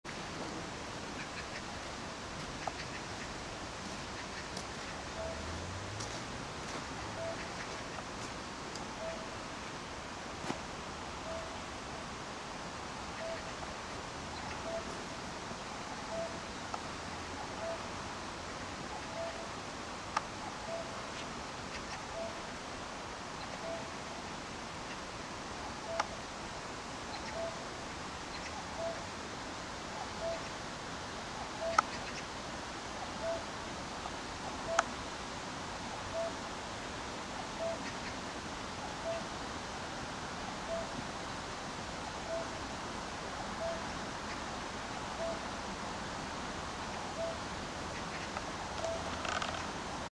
sony DEV-5 test 동막천 물고기들
sony DEV-5 test

경기도 용인시 수지구 동막천에서 살고 있는 물고기들..